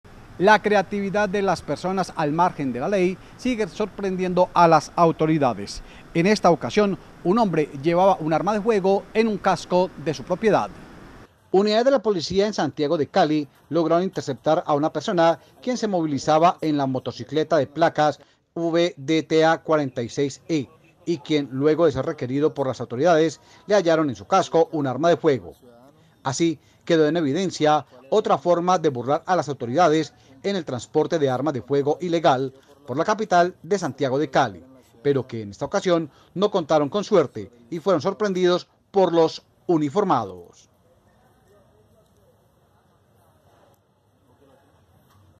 Transportaban arma de fuego, en un casco
Unidades de la policía en Santiago de Cali, lograron interceptar a una persona, quien se movilizaba en la motocicleta de placa VDT 46E, y quien luego de ser requerida por las autoridades, le hallaron en su casco un arma de fuego.

Así quedó en evidencia, otra forma de burlar a las autoridades, en el transporte de arma de fuego ilegal, por la capital Santiago de Cali, pero que, en esta ocasión, no contaron con suerte, y fueron sorprendidos por los uniformados.